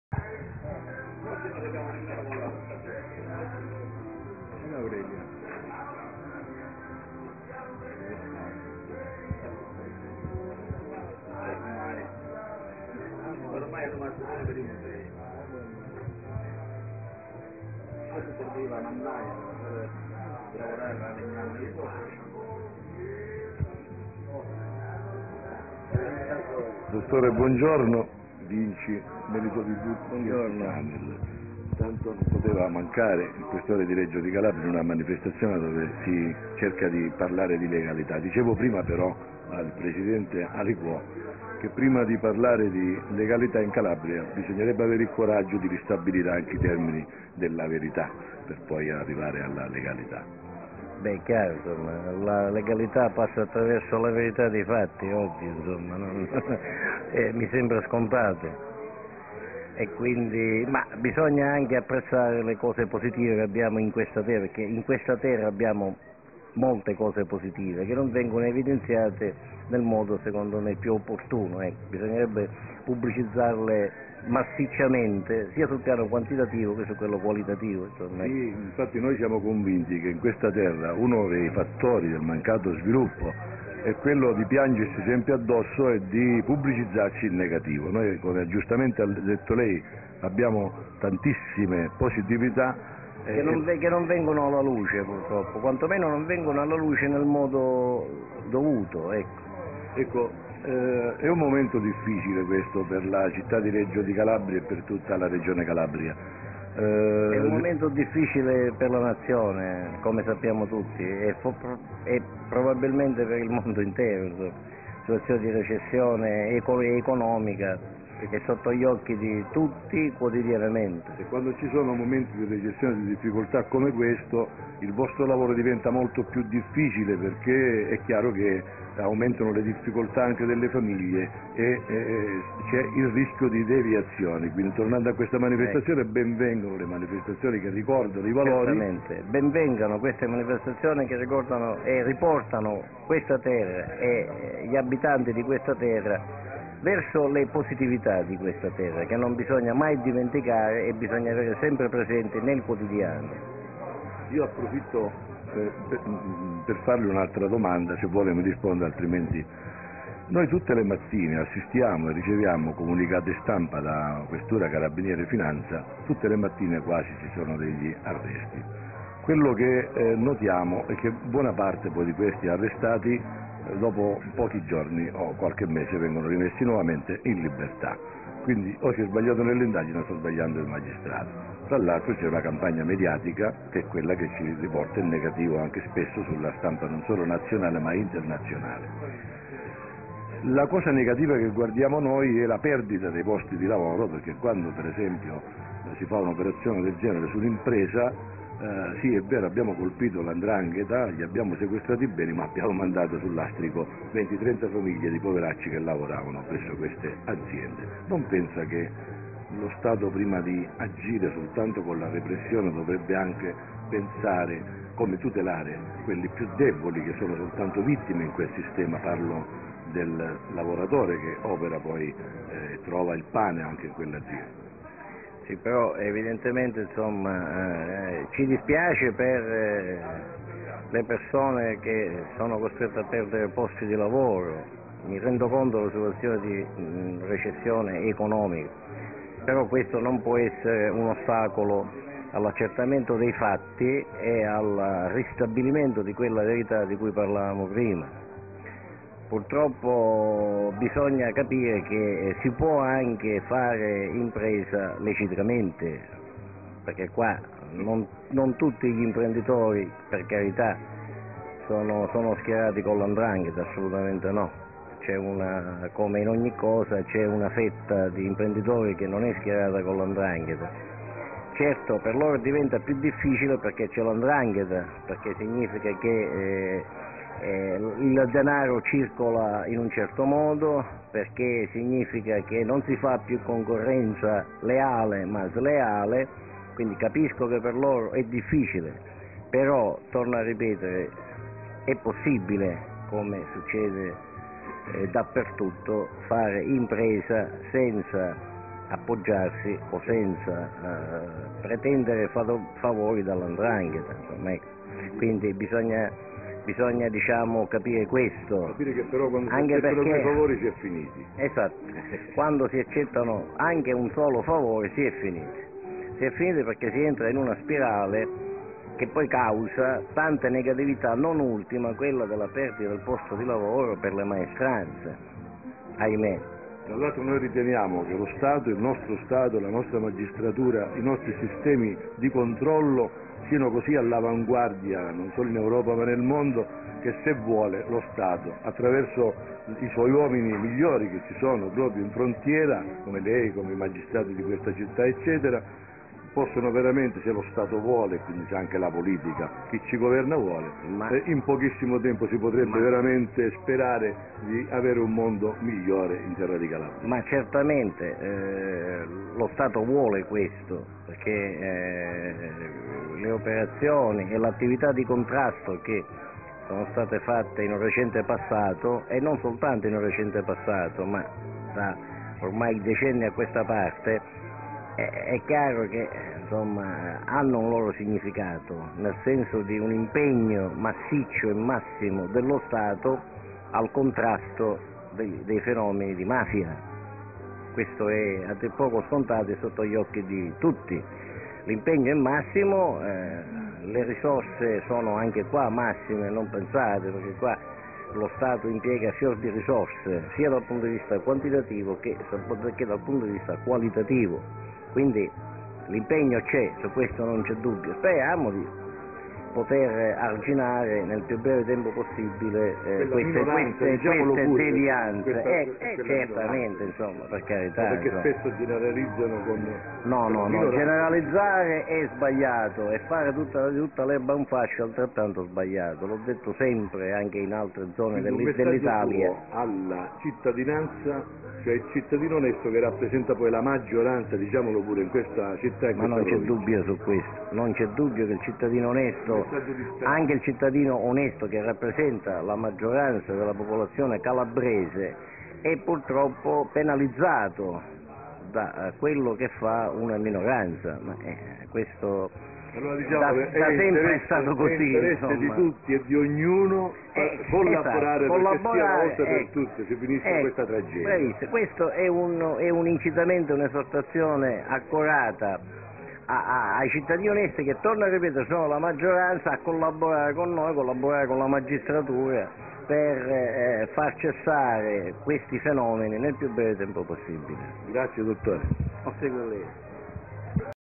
Il dott. Guido Longo Questore di Reggio Calabria alla Manifestazione " La Legalità è vita"
Reggio Calabria, 22 settembre 2012 - Inaugurazione Manifestazione " La Legalità è vita", 22 - 23 e 24 settembre 2012, Piazza Castello di Reggio Calabria.
Il dott. Guido Longo Questore a Reggio Calabria intervistato dal direttore di Melitotv Domenico Vinc